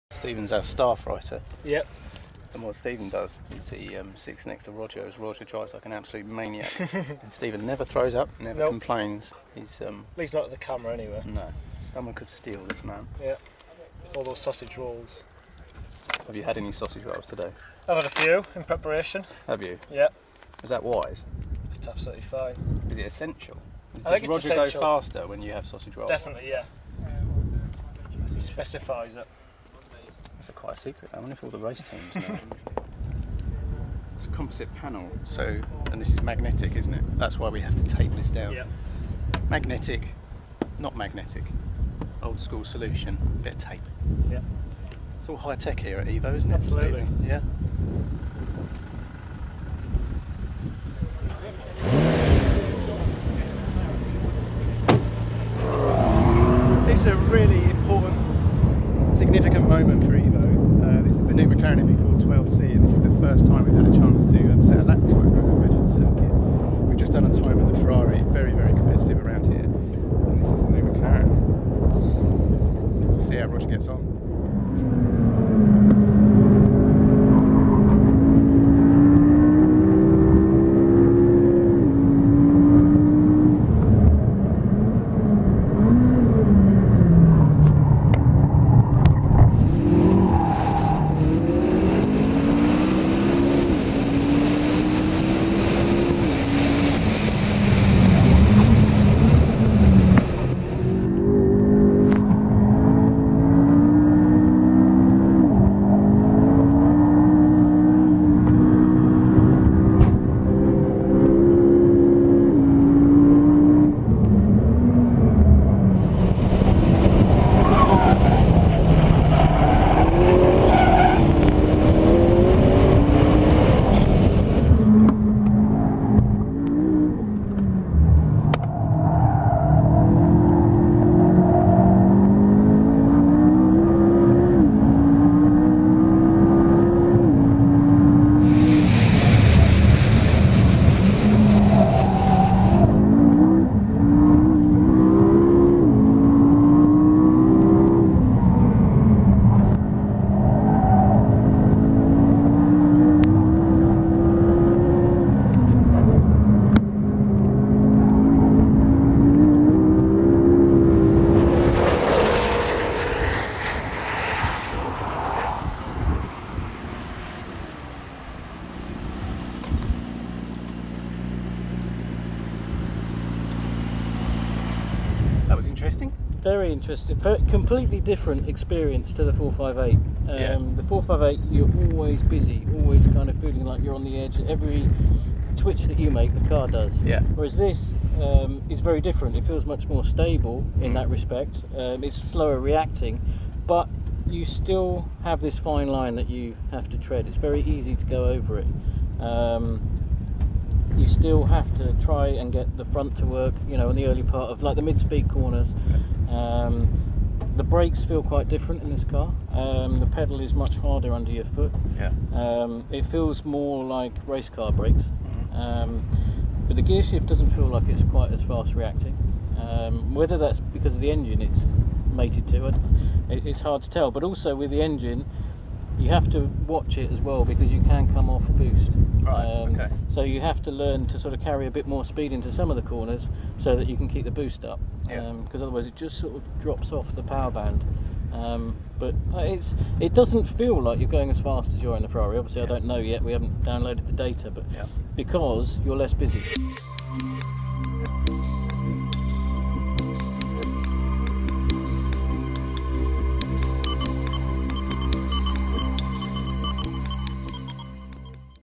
McLaren MP4-12C track test
3w.evomagazine.gr
Ενάντια στο Χρονόμετρο
Η McLaren MP4-12C κατευθύνεται στο Bedford Autodrome για μερικούς χρονομετρημένους γύρους στο Δυτικό Σιρκουί. Είναι σοβαρή υπόθεση όπως θα διαπιστώσει το evo. Διαβάστε περισσότερα στο evo #104.
Against the Clock
McLaren MP4-12C head to the Bedford Autodrome for timed laps of the West Circuit. It's serious business, as evo discovers